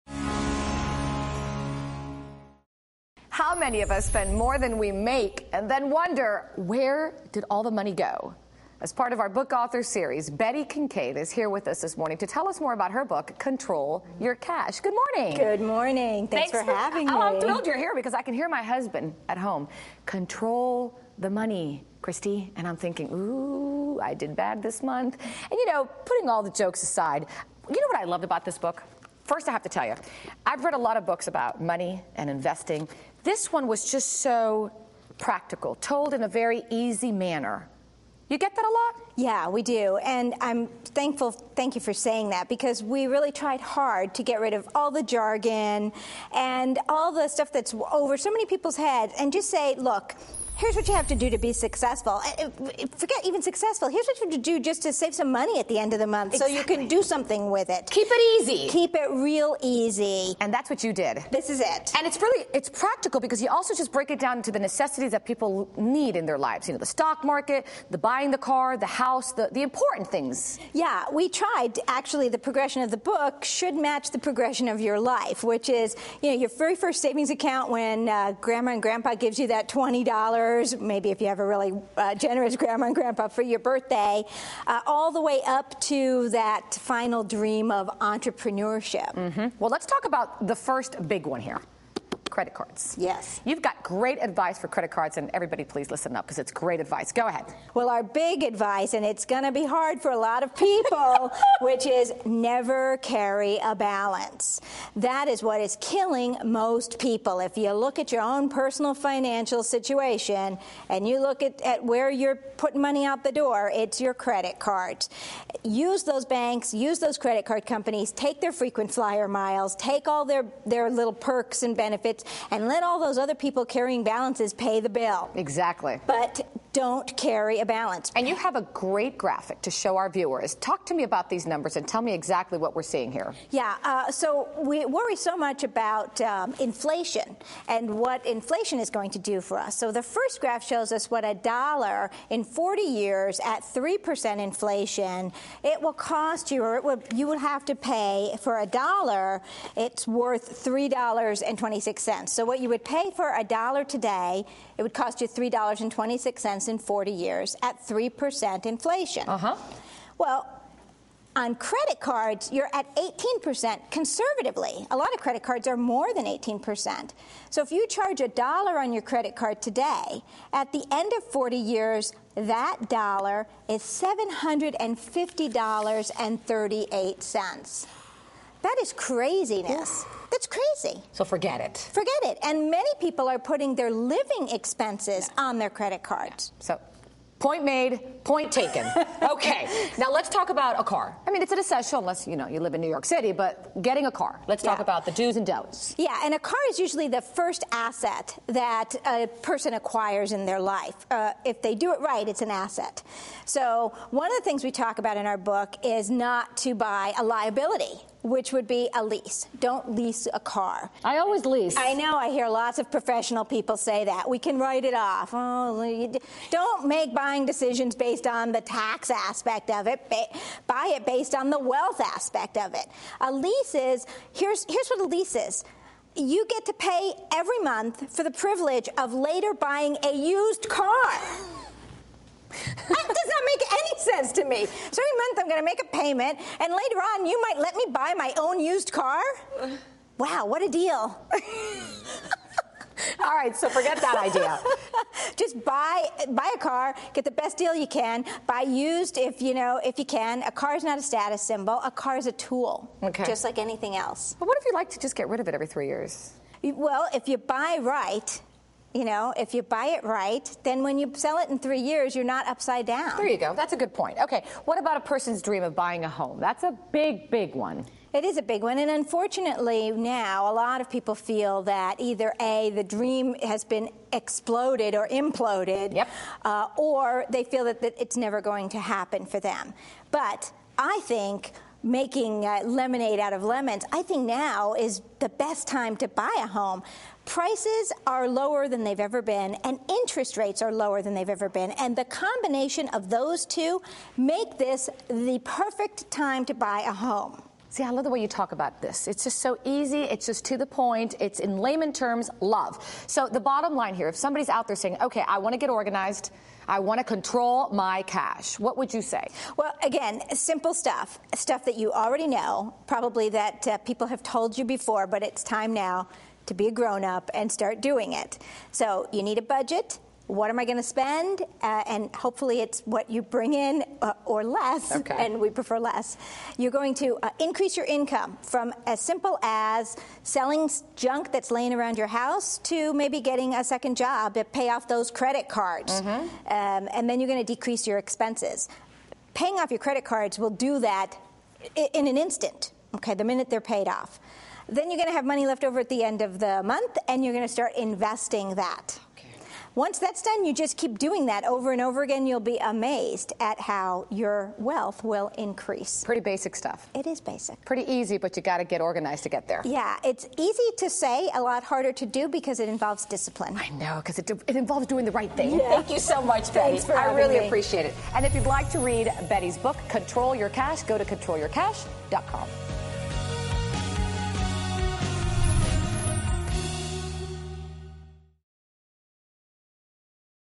Learn the fundamentals of finance
Author Betty Kincaid talks about the fundamentals of finance in her book "Control Your Cash."  She joins us today and reveals how best to save, invest and hold onto wealth.